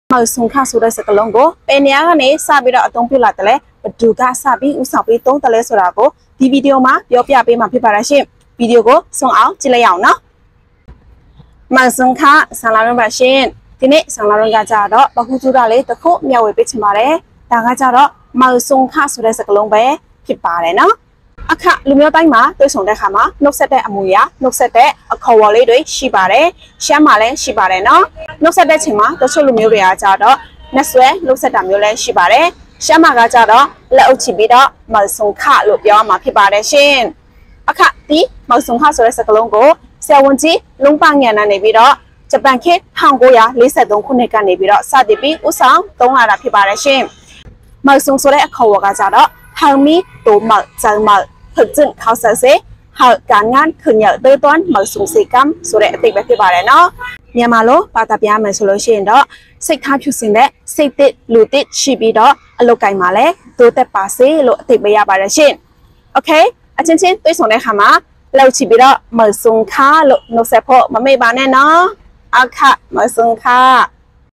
“မိုင်ဆုန်ခ” ဆိုတဲ့စကားလုံးကို ဘယ်သူစတင်ပြောသလဲ ??
“မိုင်ဆုန်ခ” ဆိုတဲ့စကားလုံးကို ဘယ်သူစတင်ပြောသလဲ ?? ဆိုတာ ဒီ Vdo မှာ ပြောပြပေးသွားမှာ ဖြစ်ပါတယ် 😍